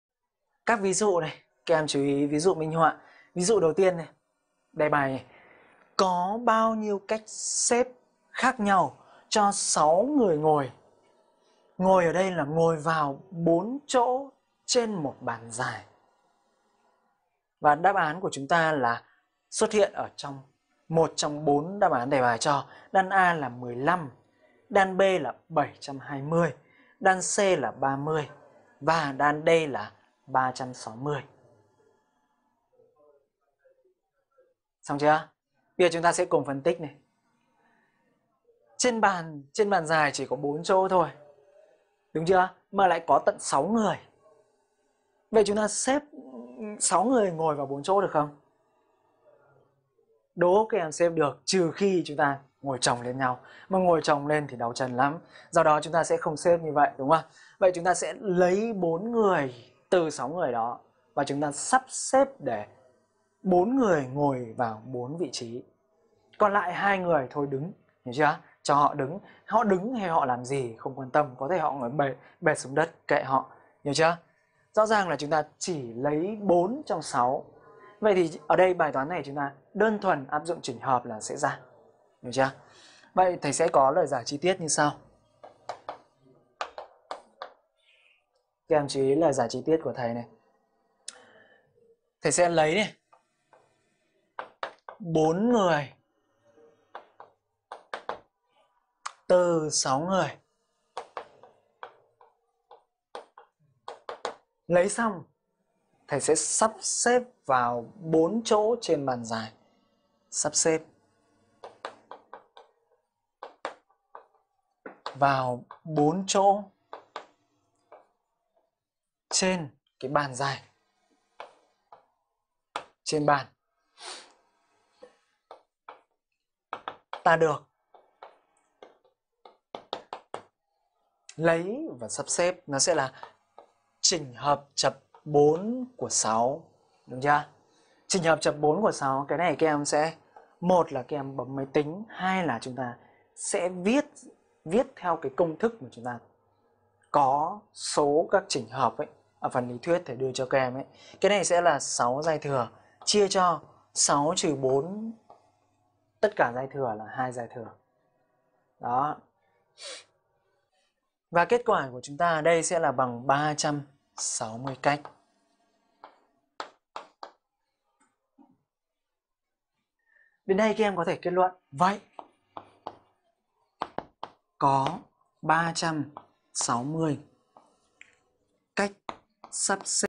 Chỉnh hợp - Tổ hợp - Xác suất - Đại số 11 VD
* NỘI DUNG BÀI HỌC:
CHỈNH HỢP
Lý thuyết

- Câu châm ngôn: trên bước đường thành công ko có dấu chân của kẻ lười biếng.
- Quan điểm: Dạy toán là dạy cách tư duy, phân tích
- Số khoá học trên trang: 03